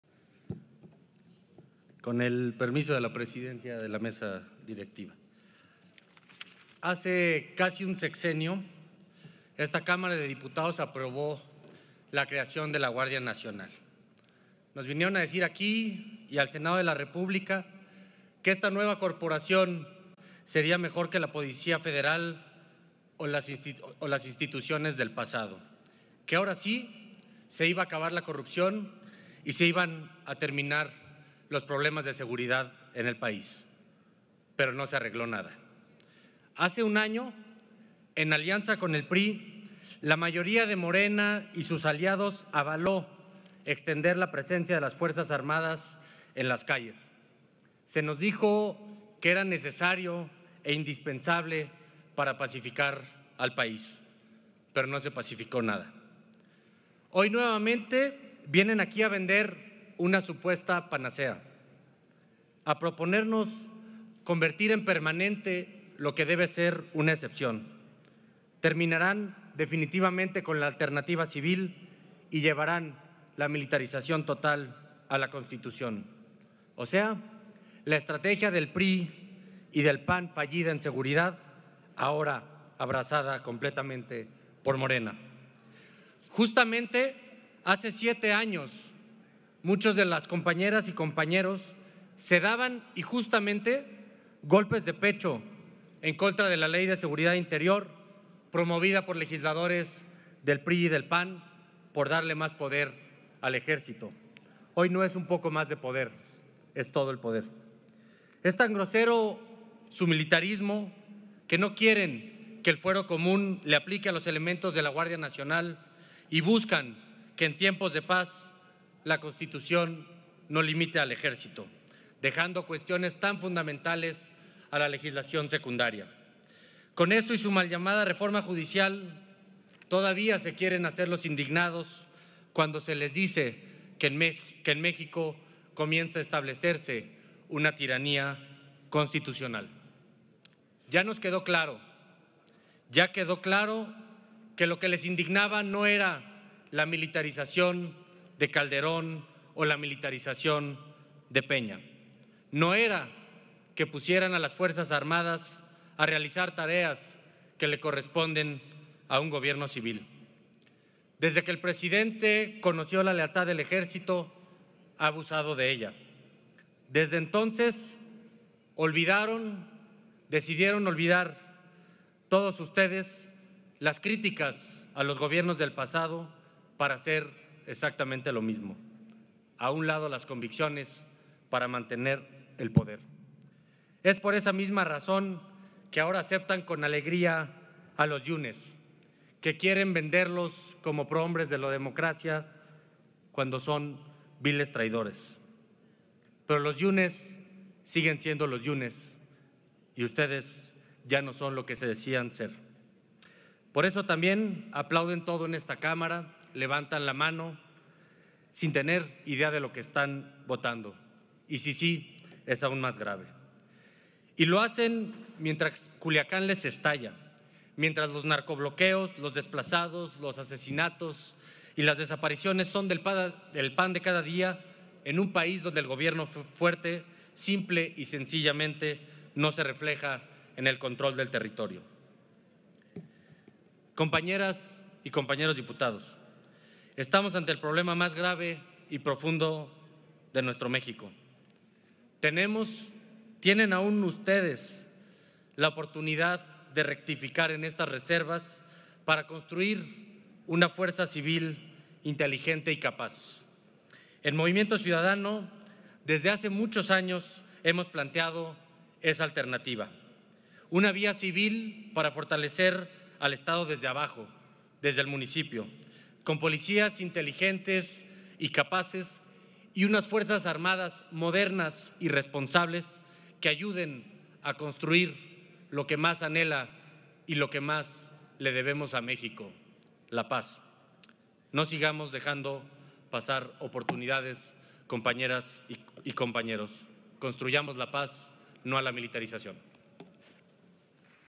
Reforma Militar | Juan Zavala
Mientras Culiacán estalla y los desaparecidos y asesinatos son el pan de cada día en nuestro país, Morena sigue apostándole a una estrategia de seguridad fallida.

Construyamos la paz. No a la militarización.

Escucha a Juan Zavala: